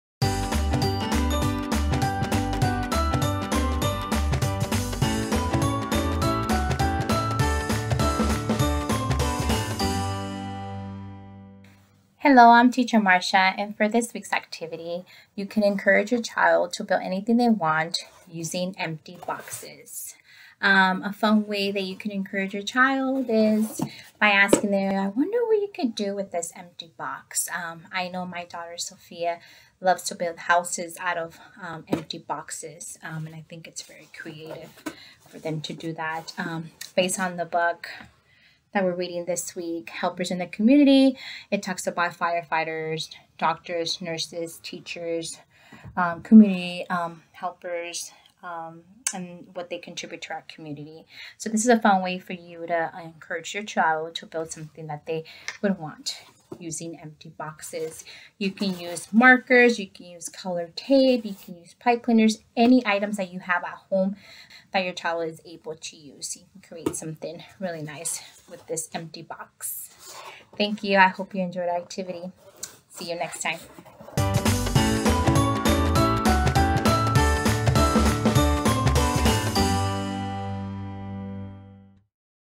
Home Activity with Teacher Marcia. Focus: Dramatic Play- Constructing using empty cardboard boxes
Home Activity with EDL 21
Teacher Marcia
Focus: Dramatic Play- Constructing using empty cardboard boxes
Lesson Plan 1/25-1/29/2021